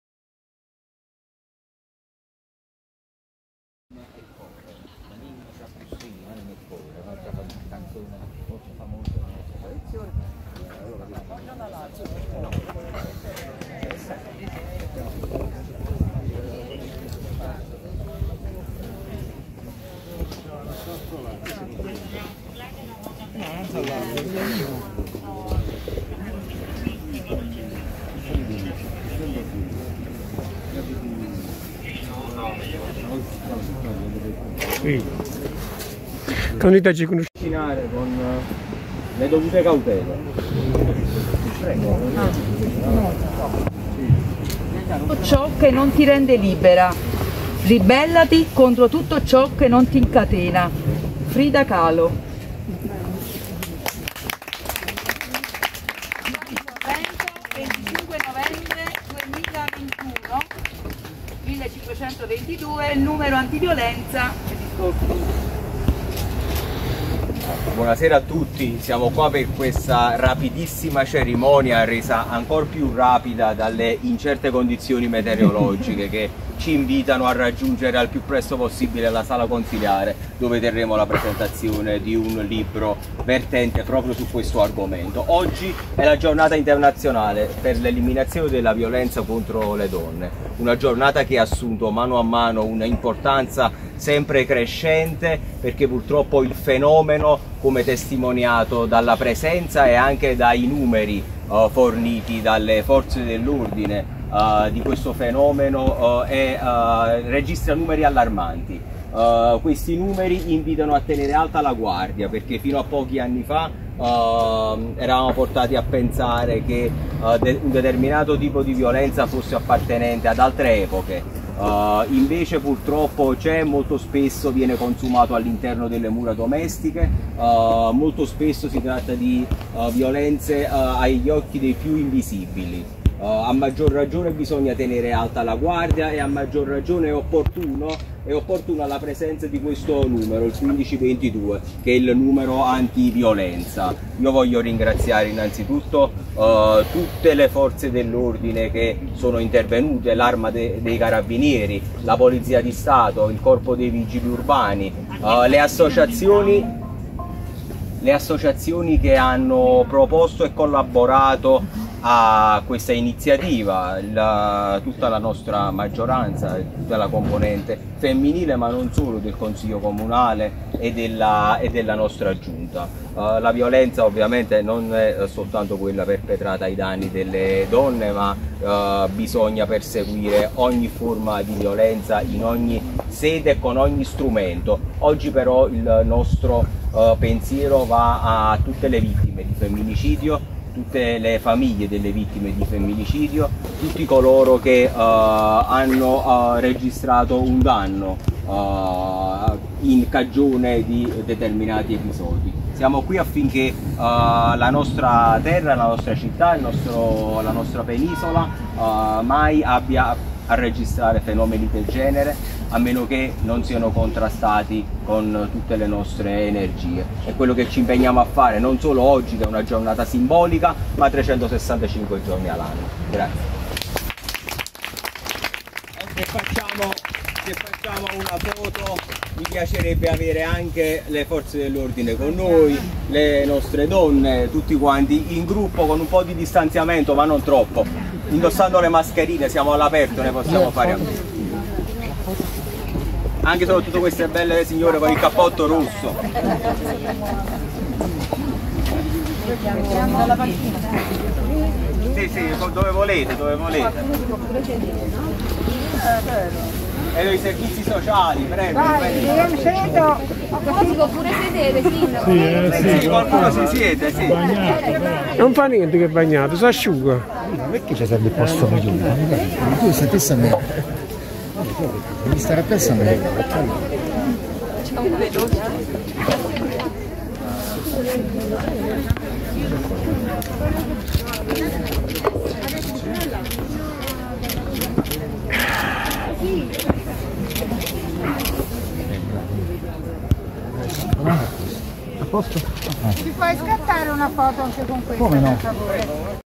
Sorrento-Panchina Rossa - Città di Sorrento Villa Comunale 21/11/2021
Città  di Sorrento  25/11/2021  Villa Comunale, in occasione della " Giornata internazionale per l'eliminazione della violenza contro le donne " posizionamento e inaugurazione della" Panchina Rossa" in memoria di tutte le donne vittime di violenza.
Intervento del Sindaco Avv., Massimo Coppola
Mda Set Dirette Web
Direzione e Regia di Michele De Angelis